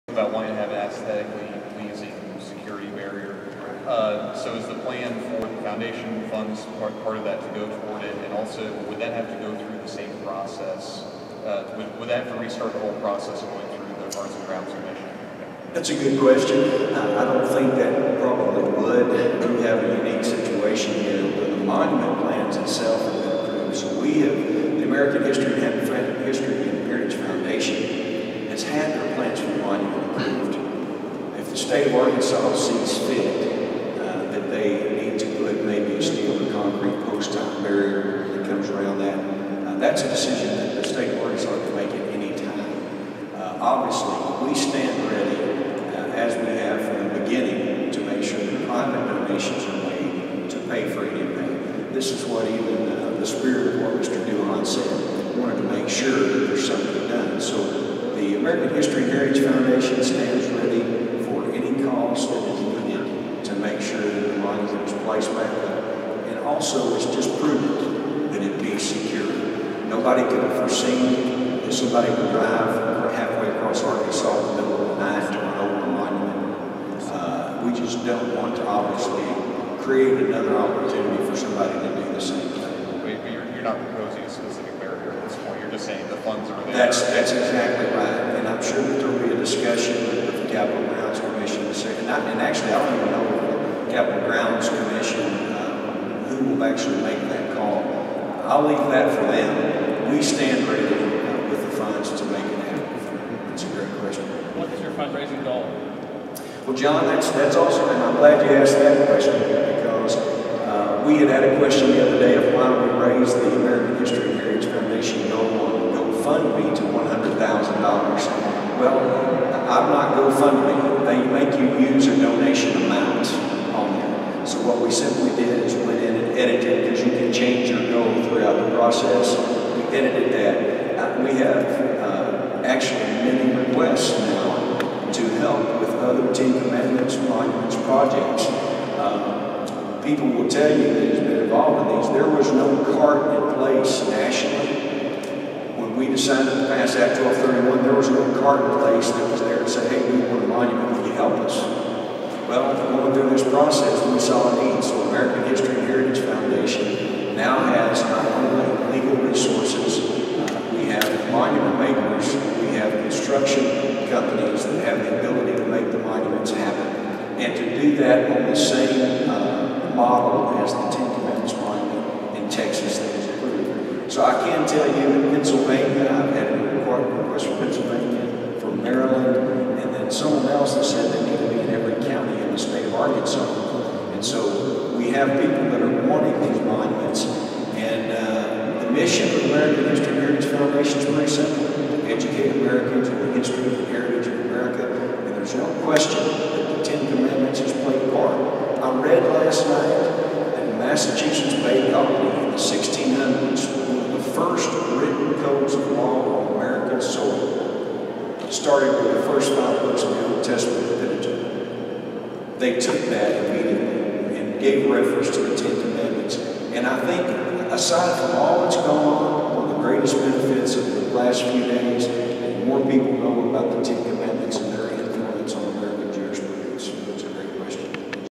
Questions the Arkansas State Capitol Ten Commandments Monument security & gofundepage
A press conference was held by film group PureFlix to present a check to The American History & Heritage Foundation, Inc. for rebuilding the Ten Commandments Monunment at the Arkansas State Capitol. The Monument was erected on Tuesday June 27th 2017 and then destroyed when Michael Reed rammed it with his car. Reed based his actions on the Separation of Church & State Phrase which many claim is in the United States Constitution.

The Monument of The Ten Commandments at the Arkansas State Capitol is supported by Arkansas State Senator Jason Rapert and was passed by the Arkansas State Legislators last session. Anti-Christian activists are proposing a "separation of Church & State wall". Other Anti-Christian activist are proposing a Satanic Monument.

Unlike the public comment meetings, there were no supporters of the Satanic Monument or the Secular Monument plans at this event. Any monument to the Arkansas State Capitol now requires approval by the Arkansas State Legislator.
The process for removing a monument also requires an act by the Arkansas State Legislator.